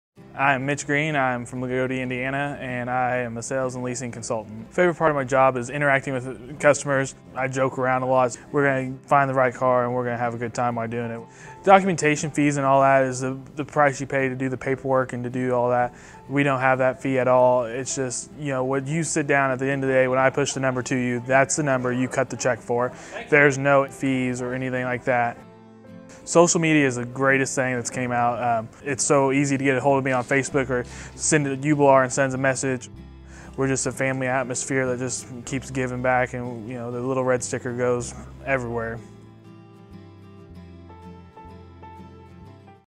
Mitch Green on Uebelhor Chevy Cadillac
Uebelhor Chevy Cadillac salesman Mitch Green explains how he makes the car buying experience fun and relaxing.